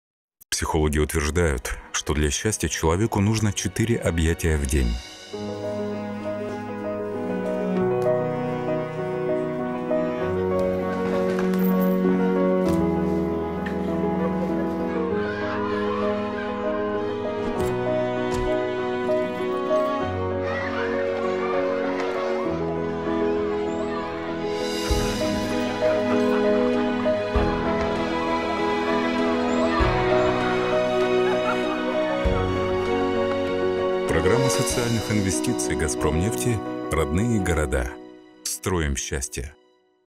Родные города. Объятия.
"Родные города" - социальная программа, придуманная для улучшения городской жизни. Если присмотритесь - ее объекты в Омске на каждом шагу: спортплощадки, библиотеки, арт-пространства. Направления разные, но они похожи в одном: там, где есть "Родные города", люди встречаются, развиваются, становятся чуть-чуть счастливее. "Объятия" - первый ролик из этой серии.
Продюсер: Александр Астахов
Сценарист: Валерия Лобанова
Режиссер монтажа: Станислав Хандеров
Операторы: Станислав Хандеров, Анастасия Васильева
Композитор: Виктория Карловски.